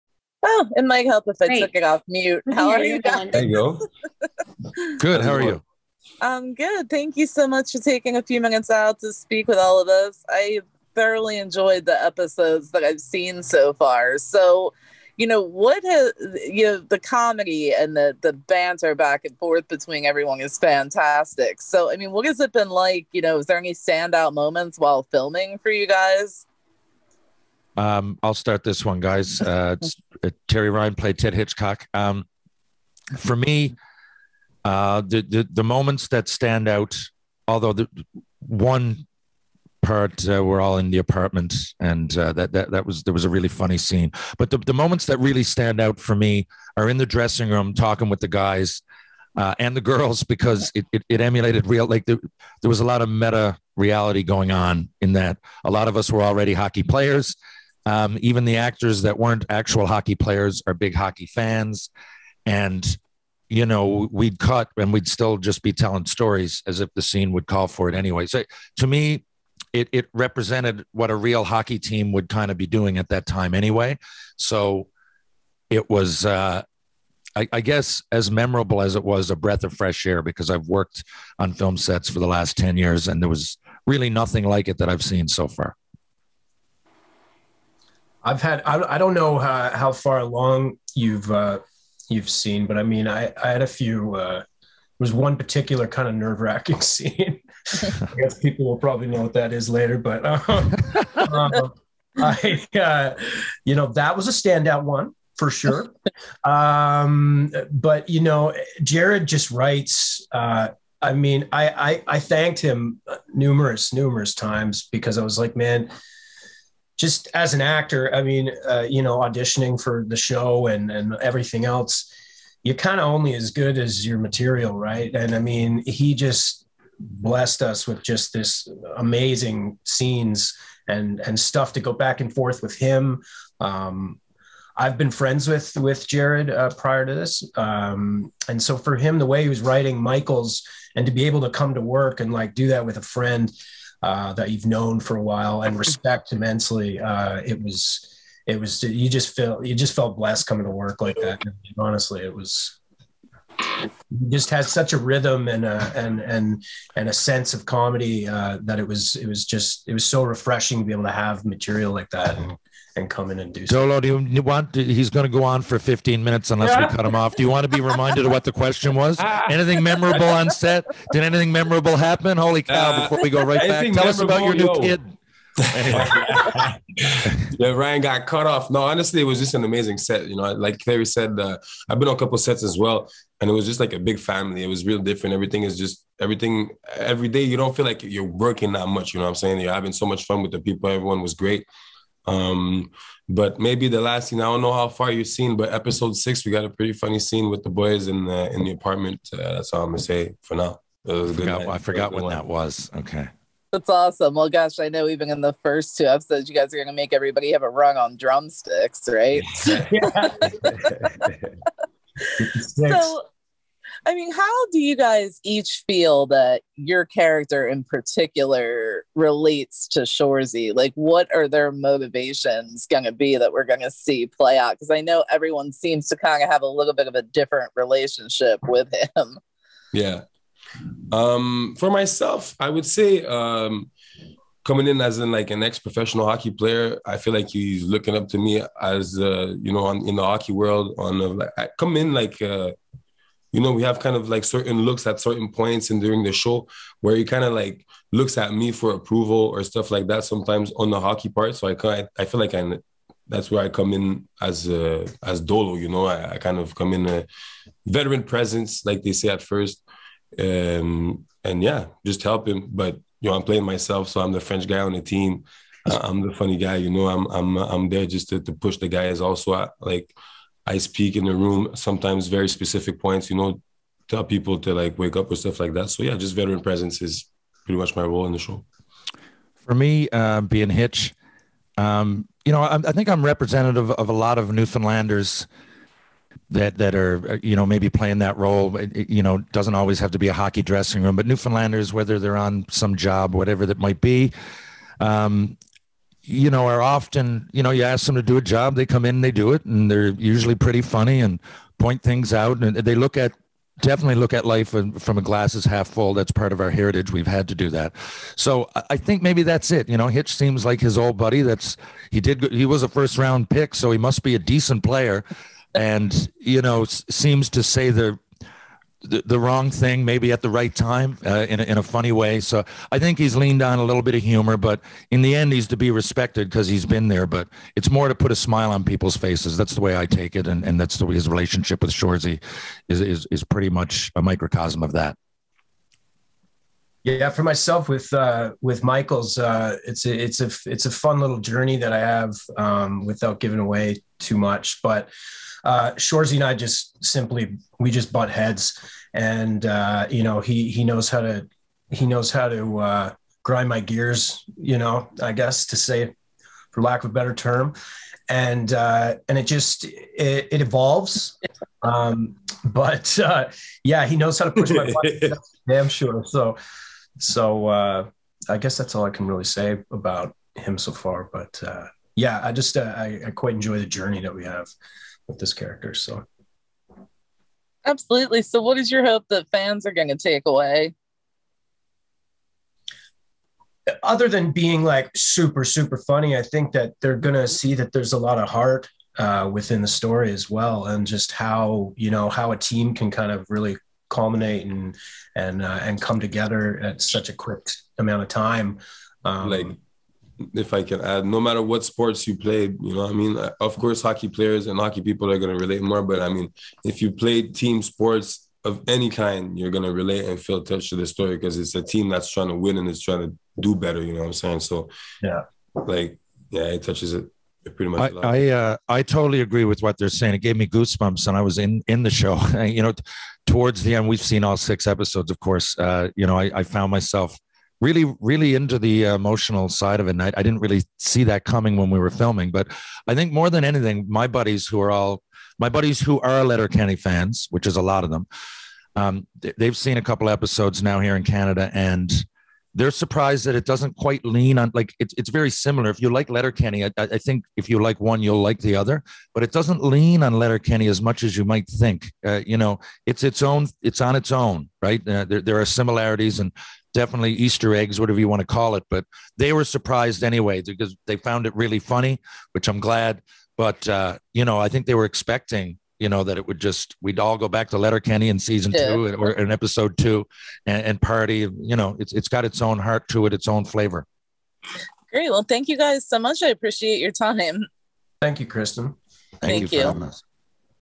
Shoresy Cast Interviews: Jonathan-Ismael Diaby, Terry Ryan and Ryan McDonell
Catching up with the cast and crew of Hulu's "Shoresy"